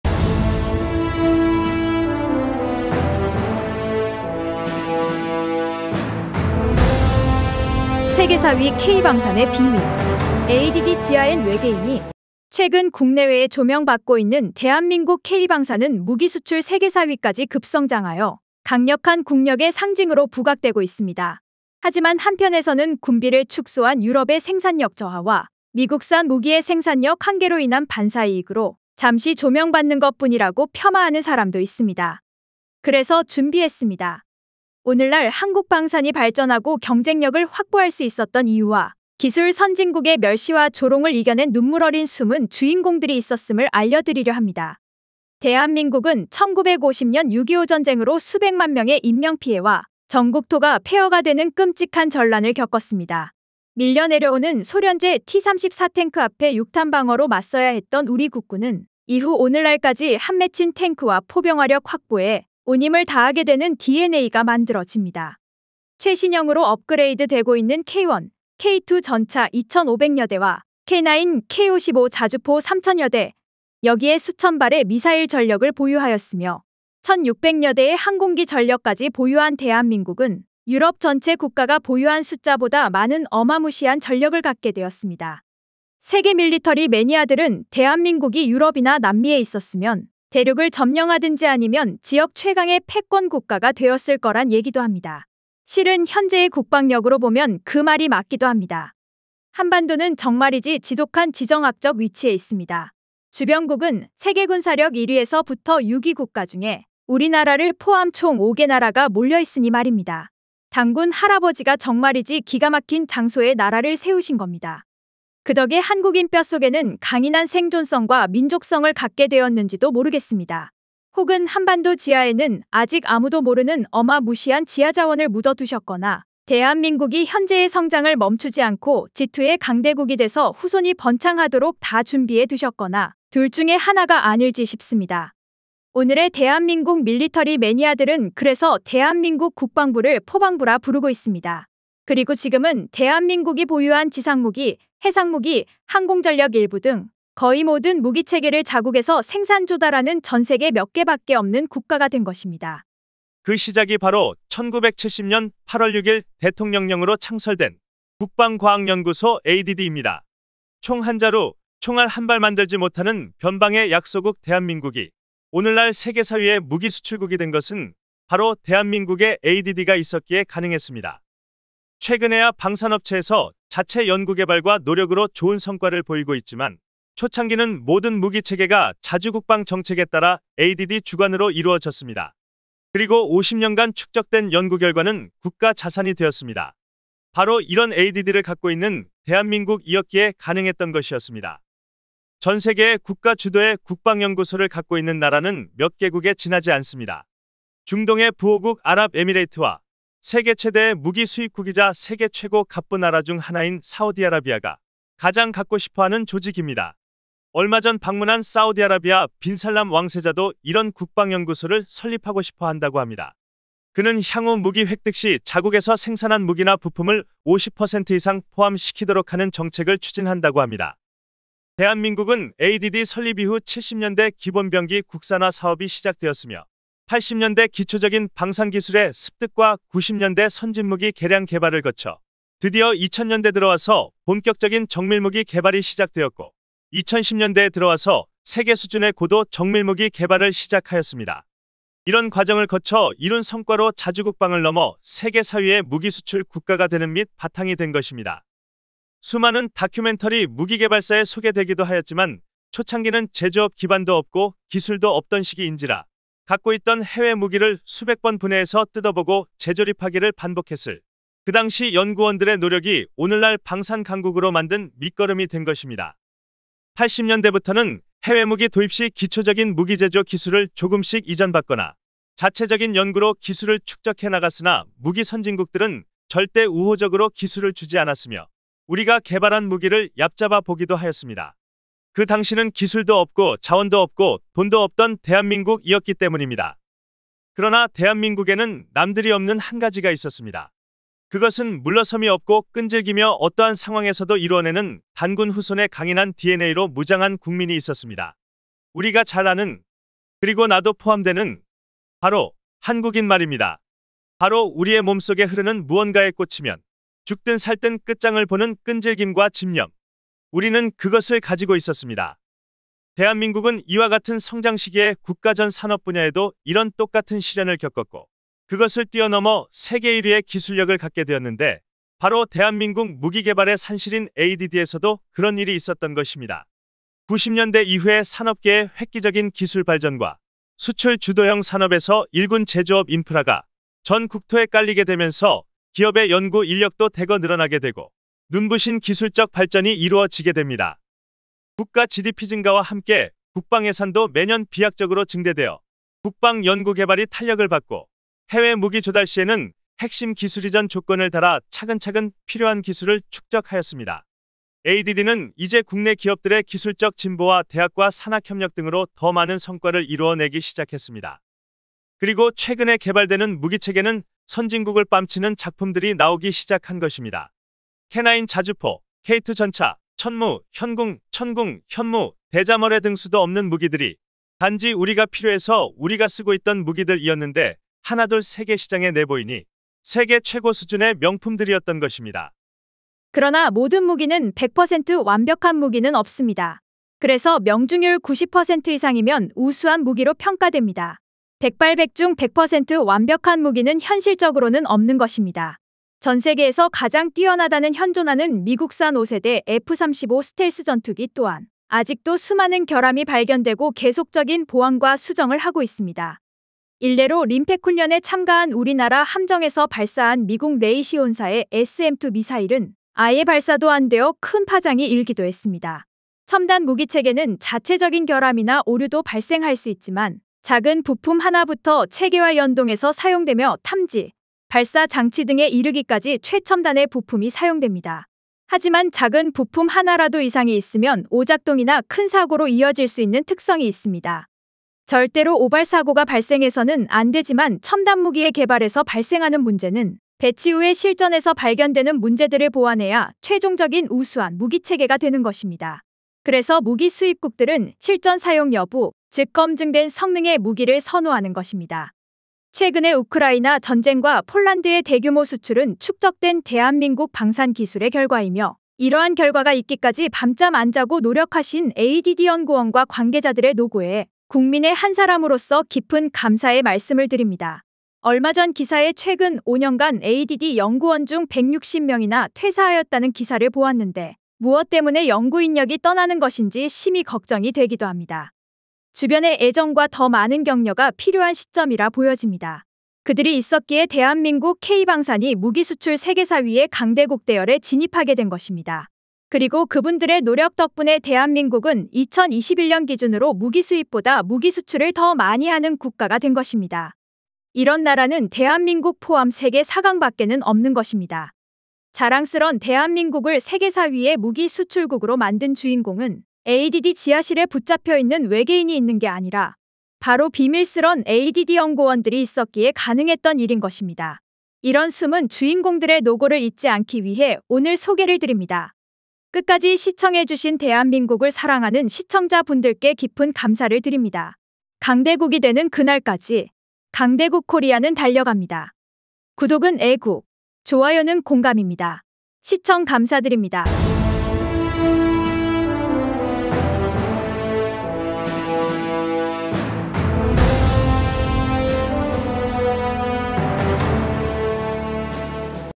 [ K방산] 빈살만이 탐내는 이것?
[ K방산의 비밀]
세계4위 무기수출 강국이 된 대한민국 K방산.
빈살만이 탐내는 이것.
오늘이 있기까지 숨은 주역들이 있었다.
ADD 지하에는 진짜 외계인이 있는 건가?
국방과학연구소.
대한민국 방위산업의 시작.
연구원의 노력.
그들의 노고에 감사 드립니다.

강대국이 되는 그날까지.. 강대국 코리아는 달려갑니다.!

대한민국 애국 체널..

강대국코리아TV